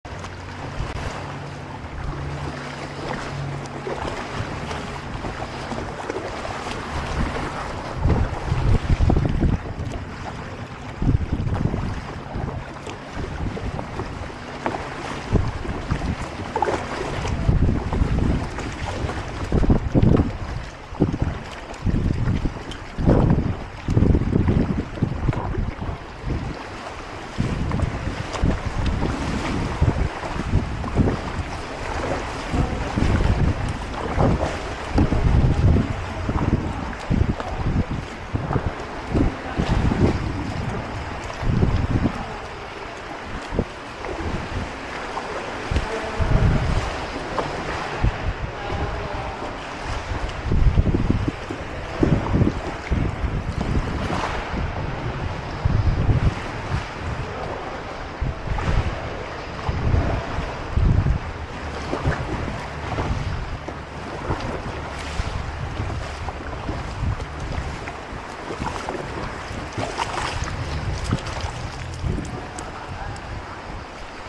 Io godo così (e scusate se è poco)
In kayak tra le grotte del Gargano